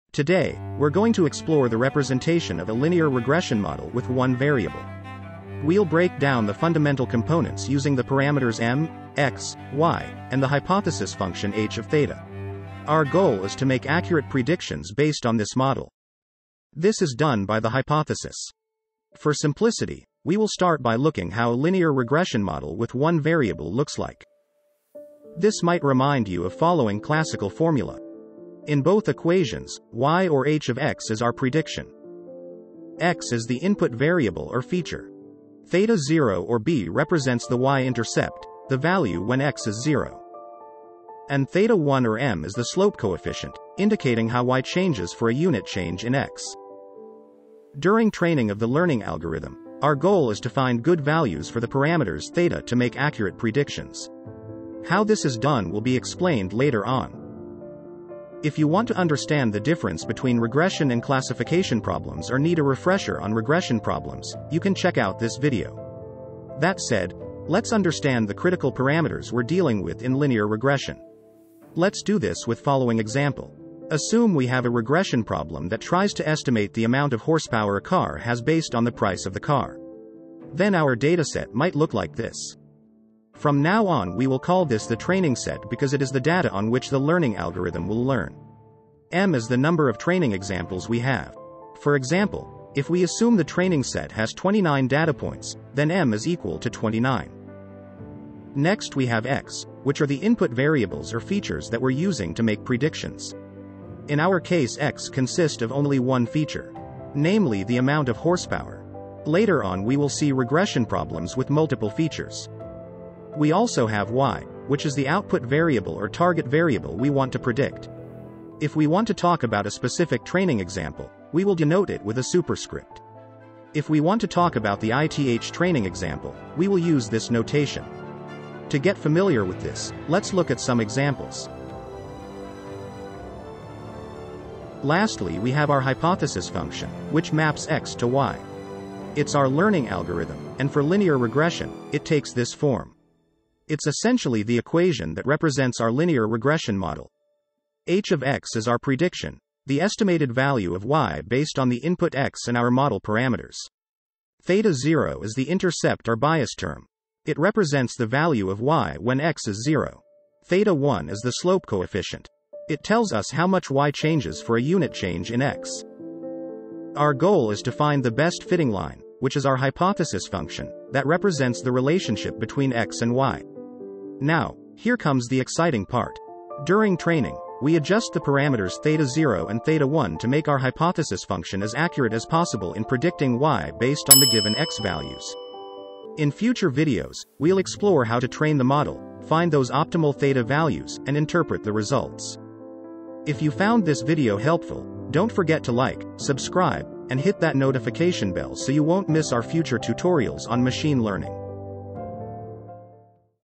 Machine Learning | LINEAR REGRESSION with One Variable | Explained in under 5 minutes!
📈 Explore the fundamentals of this essential machine learning concept, from the simple Y = mx + b equation to the power of accurate predictions. Whether you're a beginner or looking to refresh your knowledge, join us on a journey through the basics of linear regression. Let's dive in!